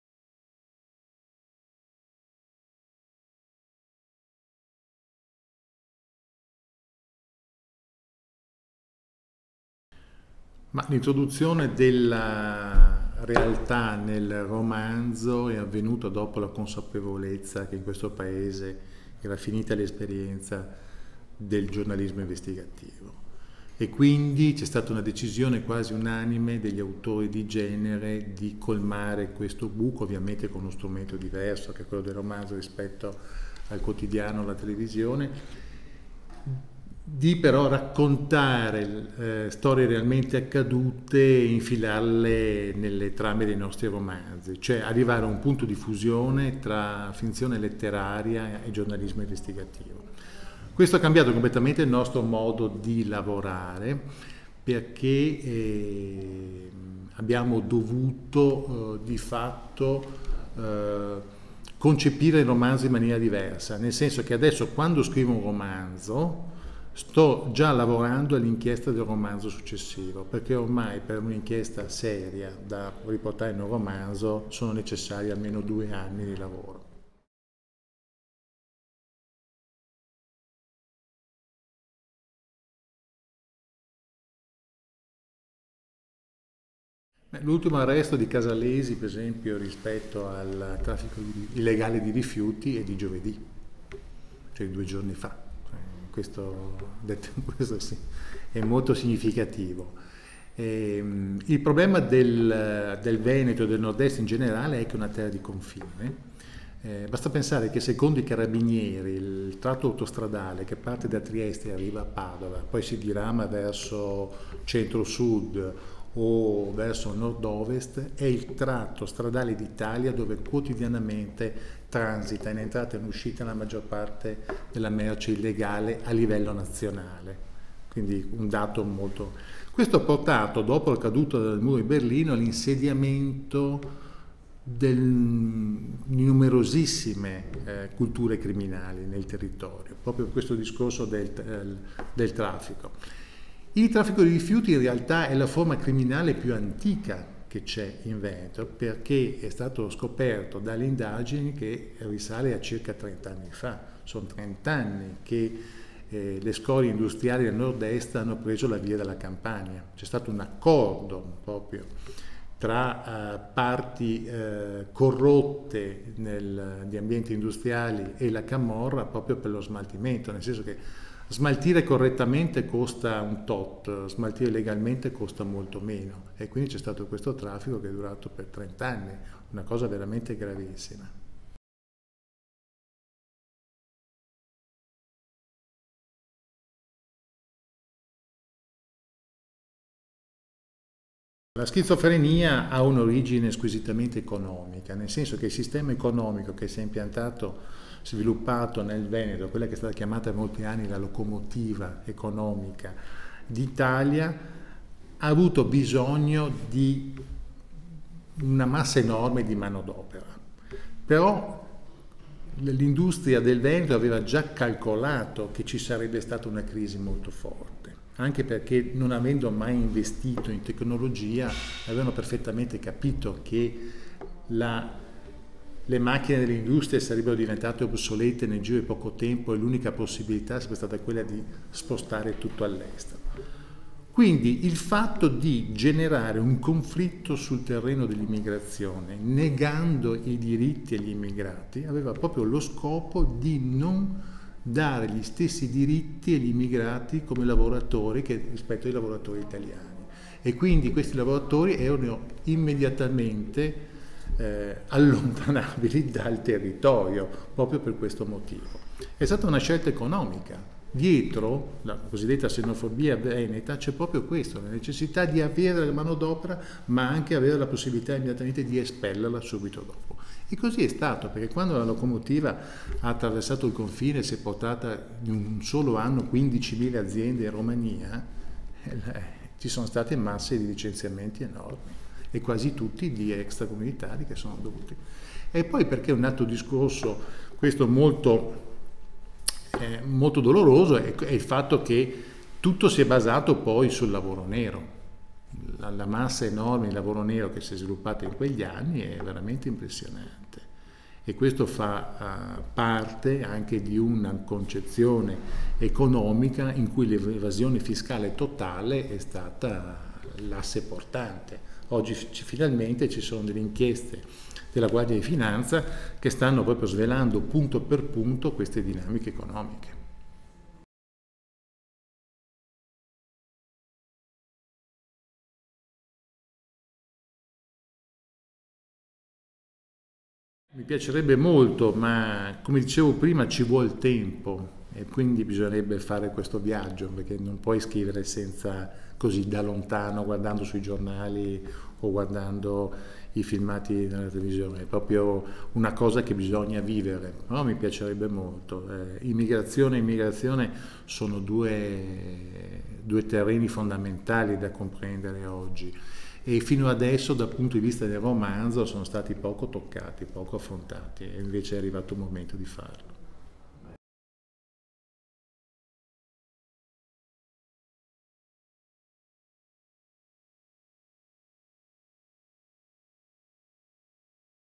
Il Nord Est schizofrenico con gli immigrati. Parla Massimo Carlotto
La realtà italiana entra nei romanzi quando finisce il giornalismo d'inchiesta. Lo scrittore padovano Massimo Carlotto, autore di numerosi noir ambientati tra nel Nord Est, racconta come e perché le questioni ambientali, il traffico dei rifiuti, l'uranio impoverito, entrano nei suoi romanzi. E spiega la schizofrenia dei veneti di fronte agli immigrati: servono come manodopera ma vengono respinti perché diversi.